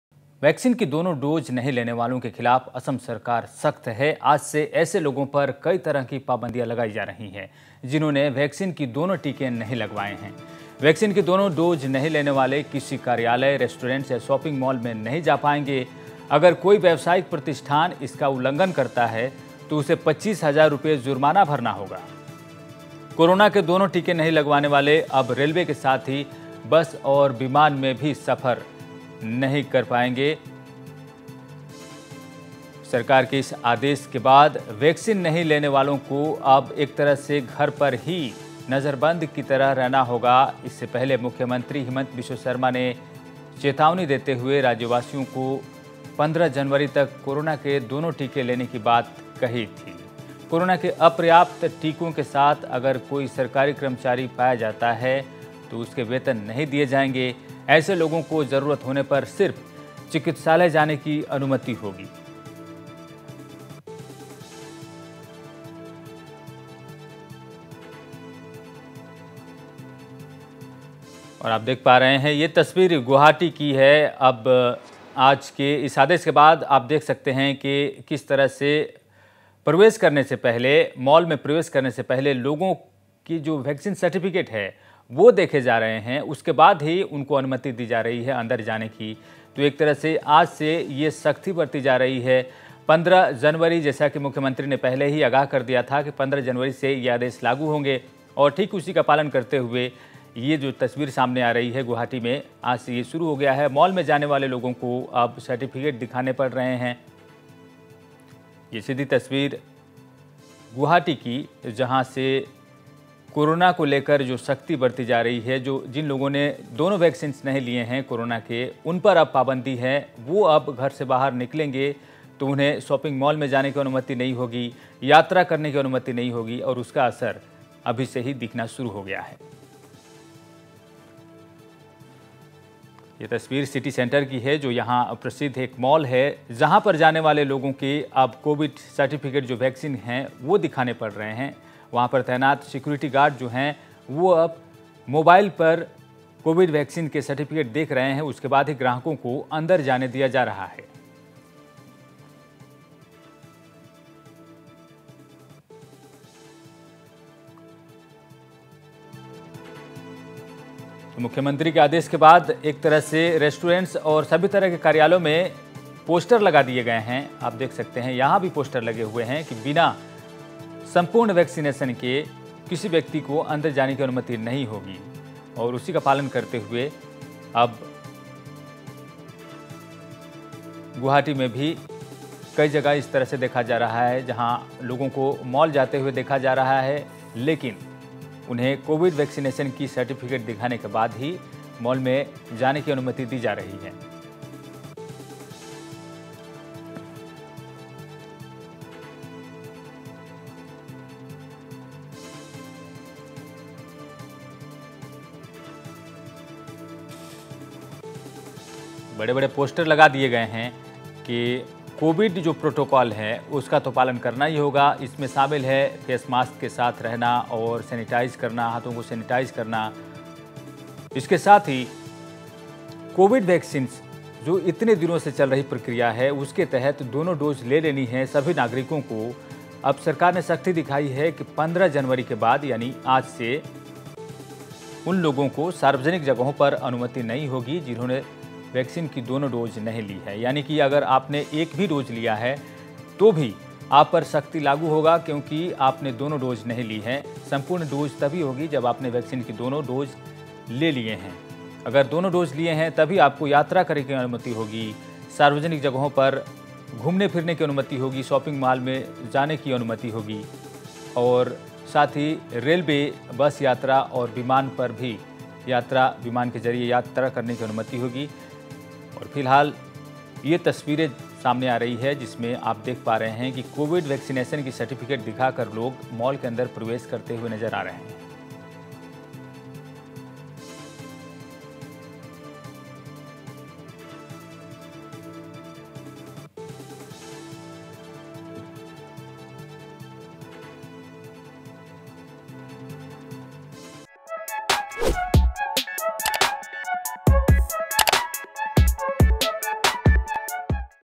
दोनों टीका नहीं लेने वालों का सार्वजनिक स्थान पर प्रवेश बंद
Assam | वैक्सीन की दोनों डोज नहीं लेने वालों के खिलाफ असम सरकार सख्त है. आज से ऐसे लोगो पर कई तरह की पाबंदियां लगायी जा रही है जिन्होंने वैक्सीन की दोनों टीके नहीं लगवाए है.